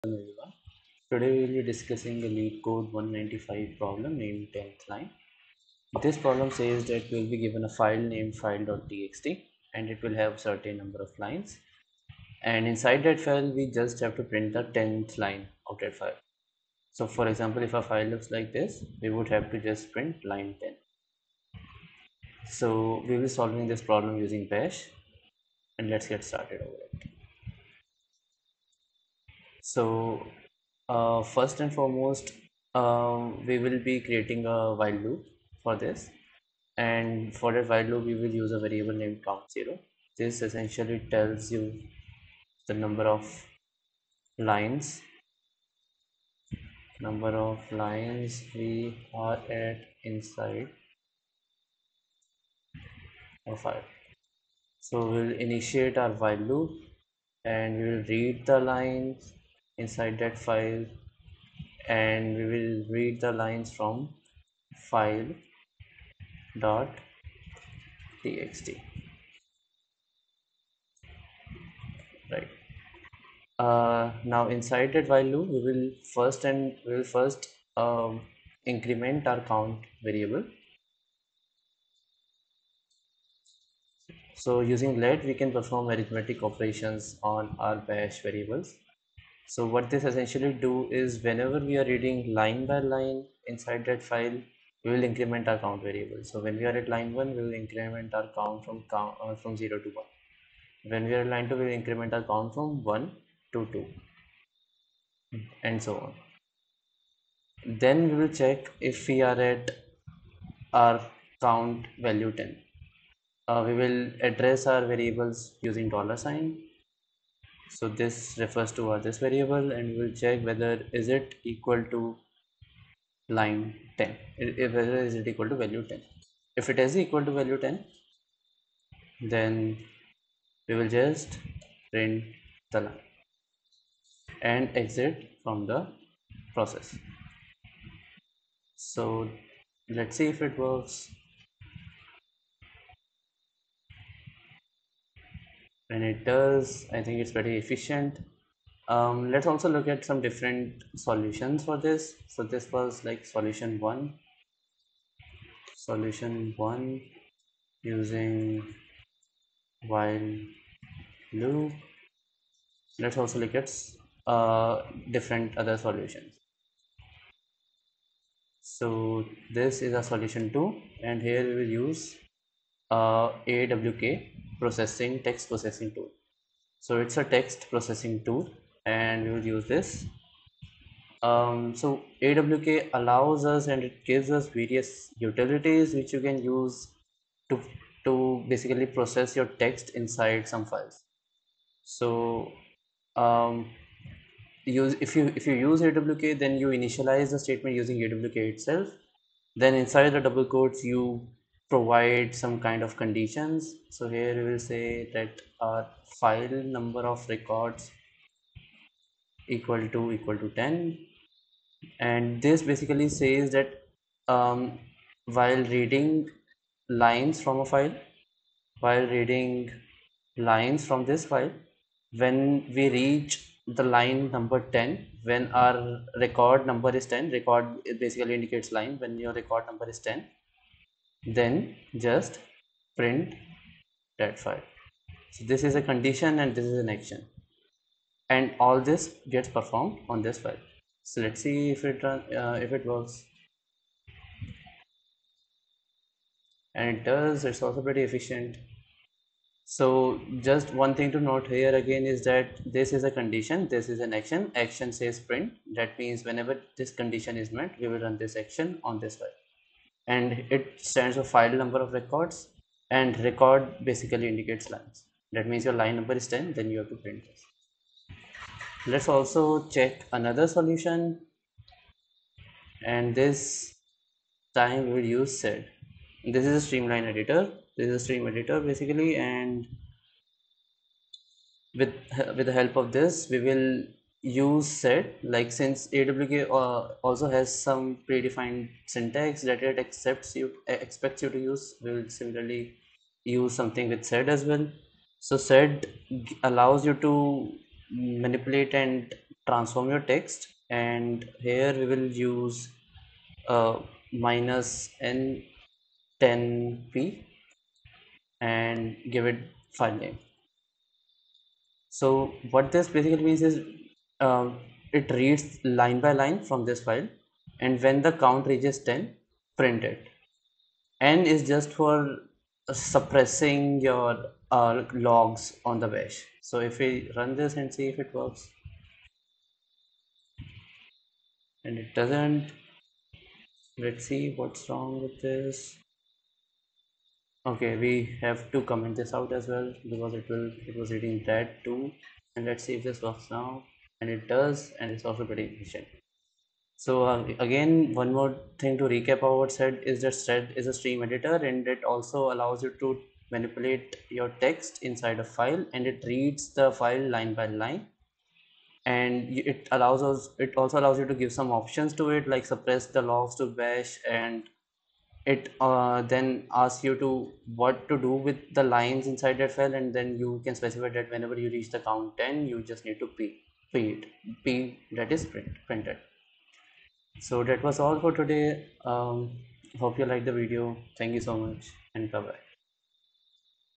Leetcode 195 - Tenth Line | Solving problems using bash
Solving leetcode problem 195 - Tenth line